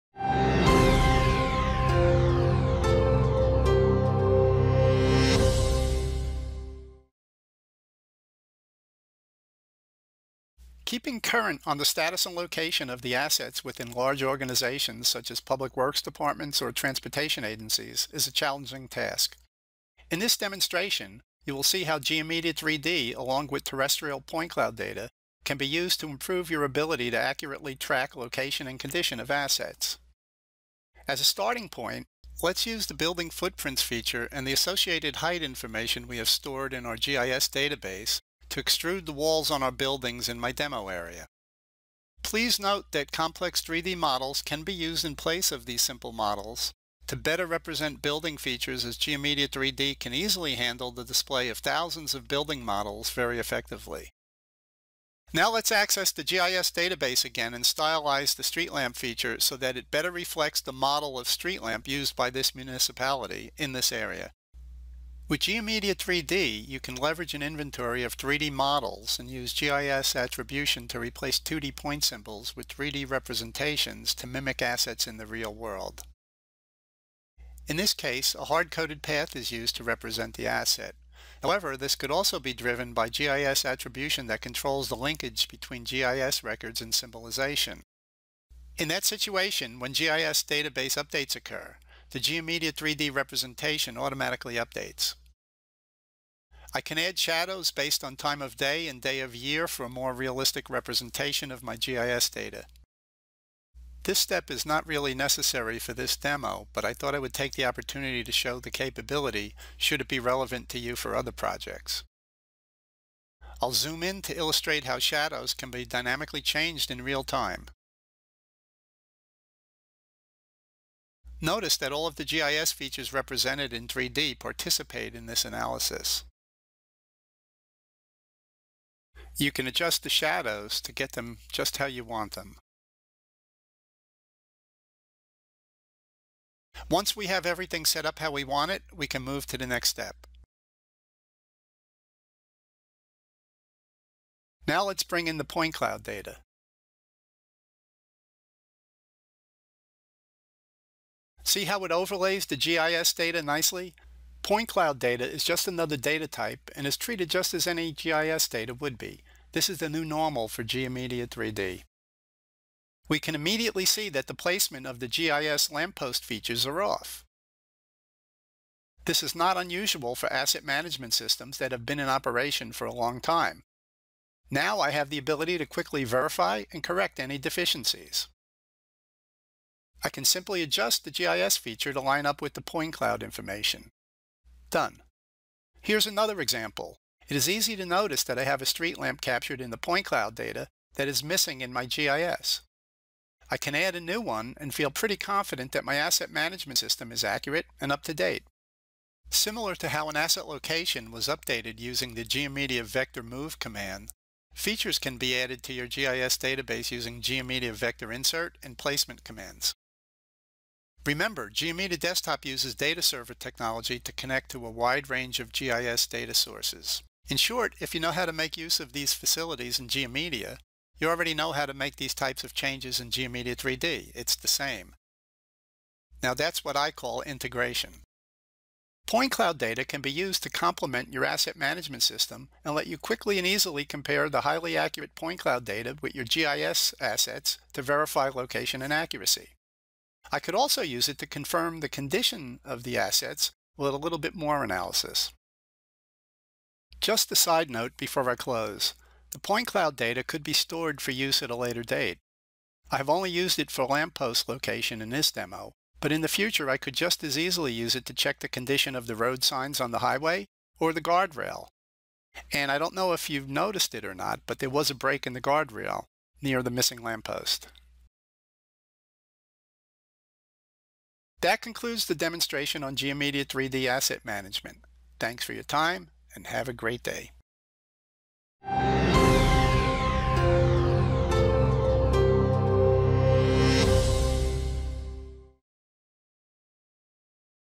GeoMedia 3D Asset Management Demo
Keeping current on the location and status of assets within large organizations is a challenging task. The increasing availability of both airborne and terrestrial point clouds combined with the ability to view them side by side with existing GIS data in a 3D view presents new opportunities for rapid, intuitive, and accurate asset management.